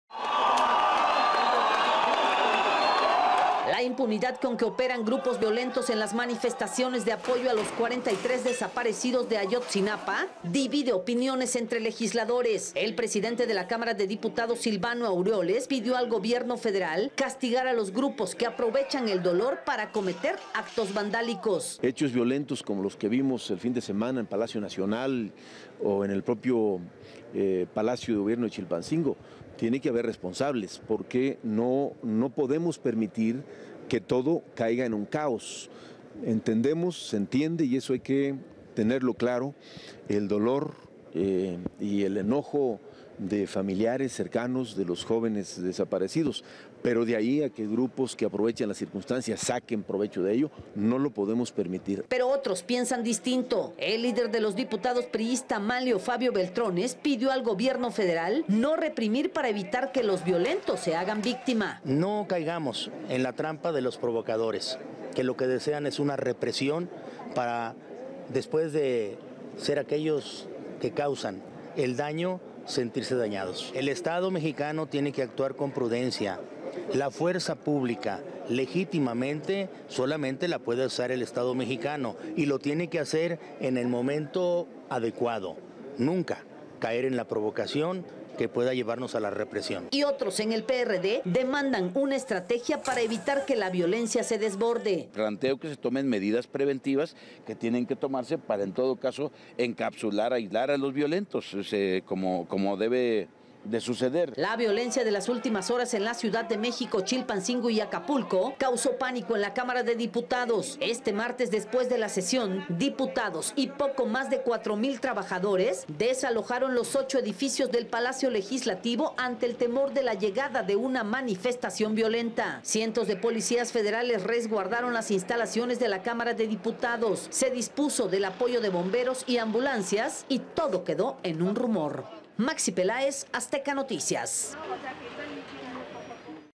Manifestaciones dividen opiniones de diputados | Noticias
Diputados tienen diversas opiniones sobre manifestaciones violentas en el país.